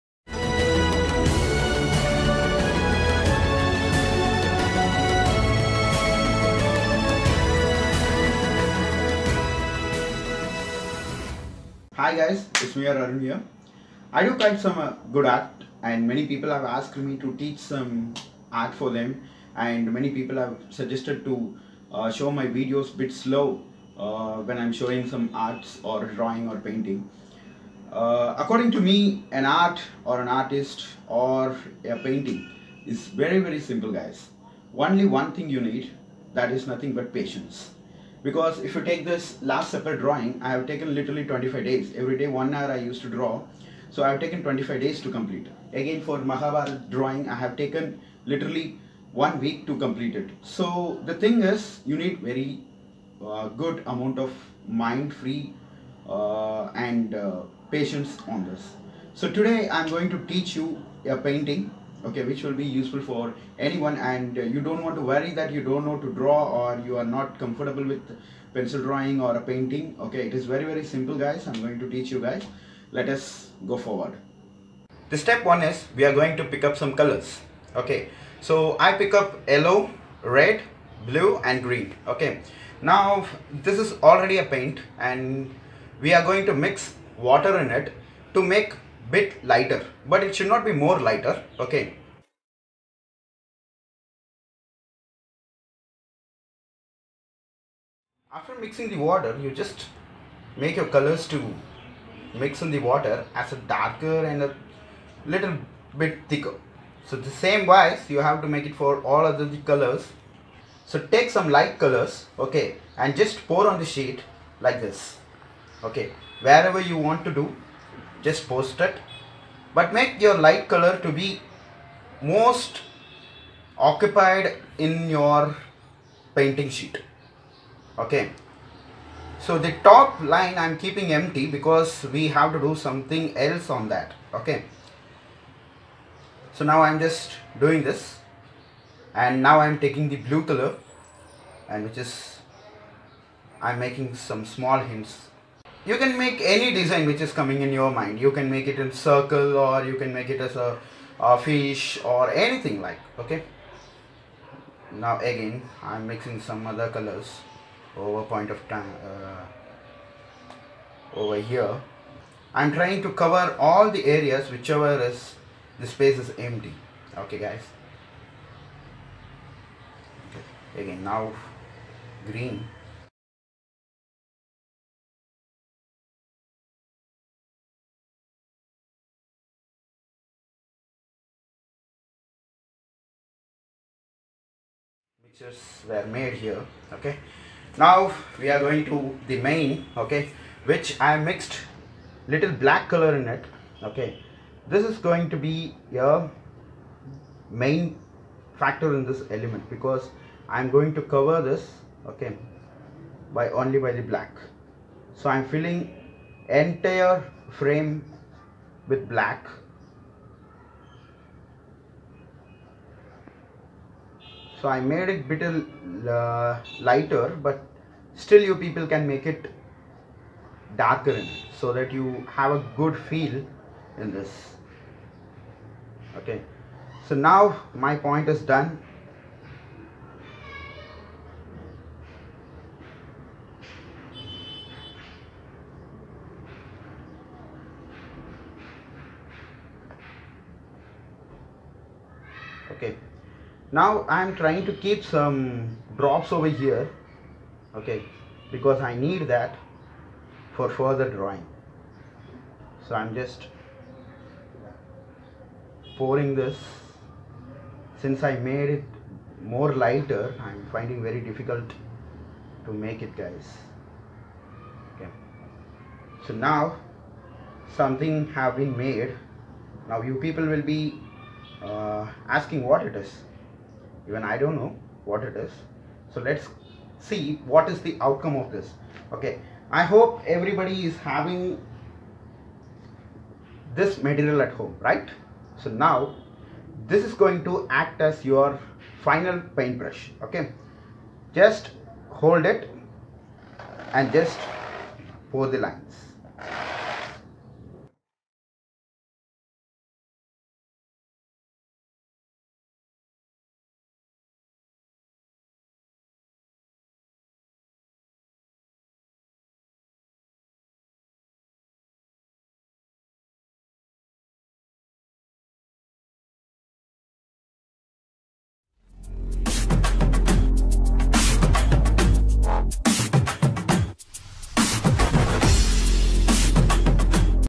Modern Art - Forest Painting - Learn to Paint in 5 Min
Modern Art - Forest Painting - Learn to Paint
Easy to draw - It takes less then 10 min to draw.